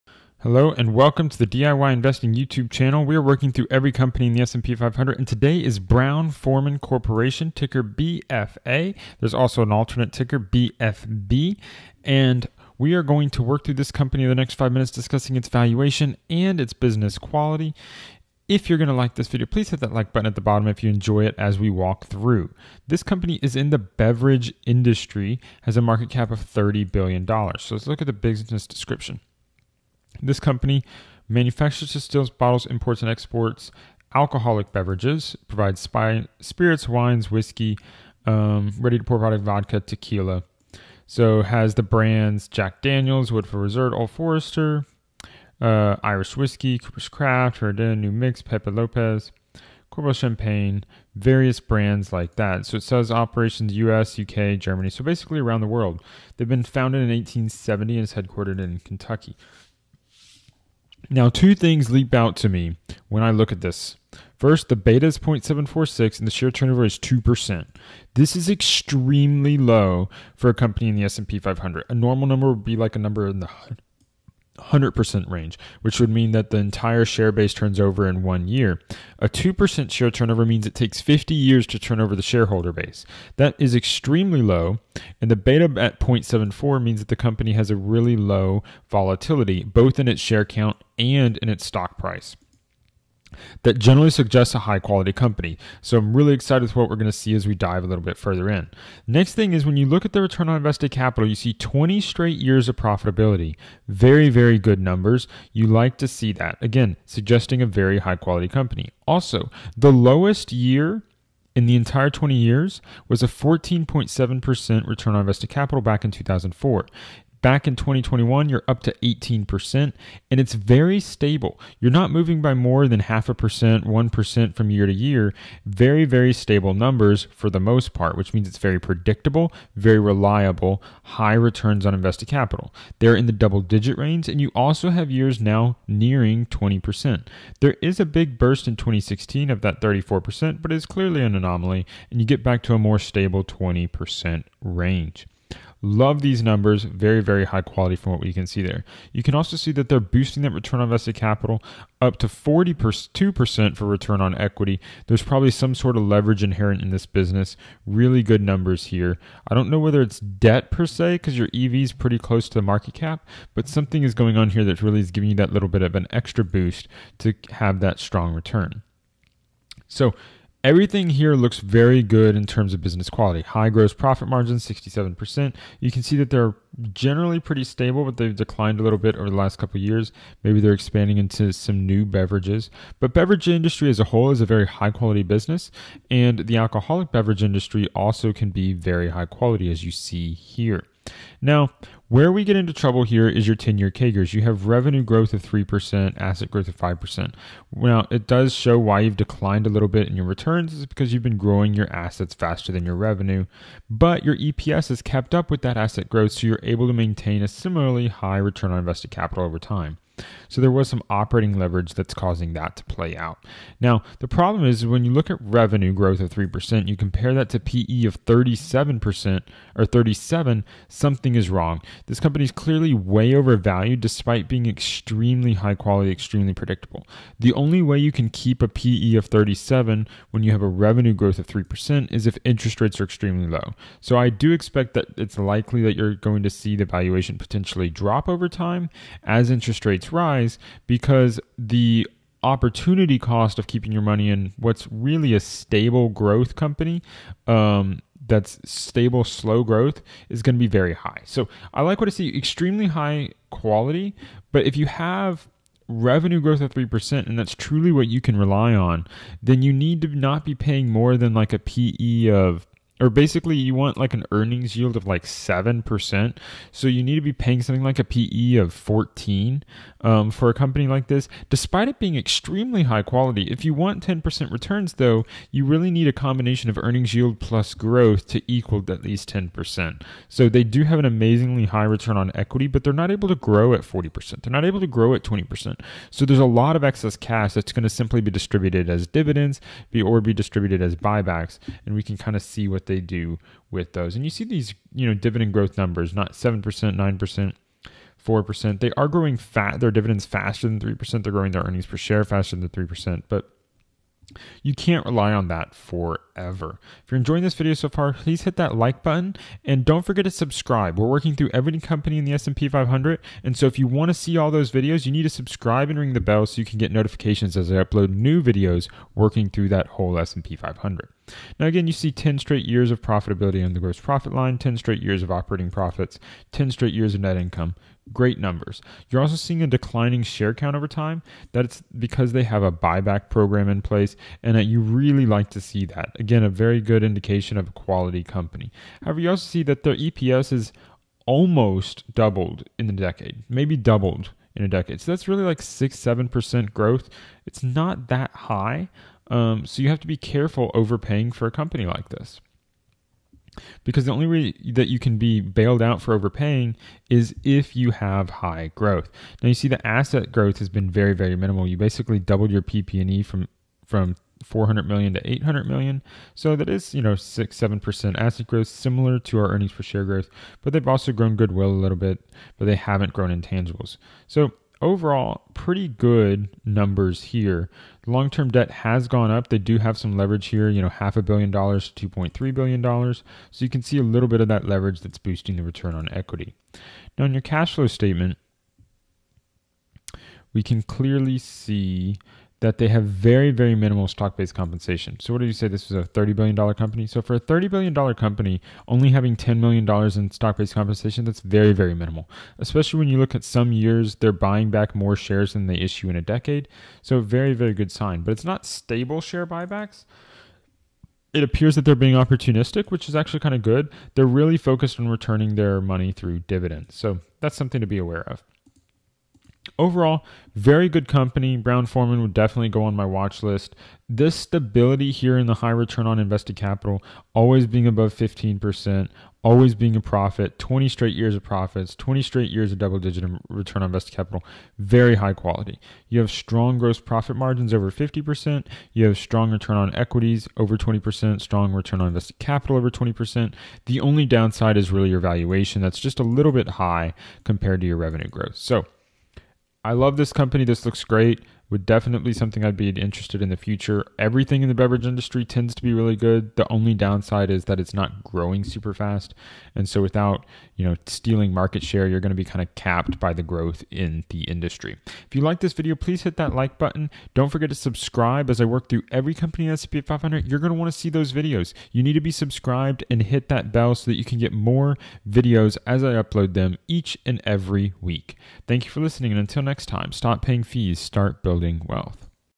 Brown-Forman Corporation Stock Analysis (7 minute First Look) - $BF.A $BF.B
(Stock Research Direct to Your Inbox)

I analyze Brown-Forman Corporation, stock ticker: $BF.A $BF.B in a short 7 minute video focused on fundamental stock analysis. My goal is to quickly determine whether this is a high-quality company worth investing in.

In addition, I provide a stock valuation analysis breaking down the current stock price compared to my target stock price for Brown-Forman stock.

QuickFS.net is used to quickly analyze the company's business description, rates of return, growth rates, and business fundamentals. I discuss the balance sheet, cash flow statement, and income statement to provide a holistic overview of the stock in five minutes or less.

Connect with Trey Henninger of DIY Investing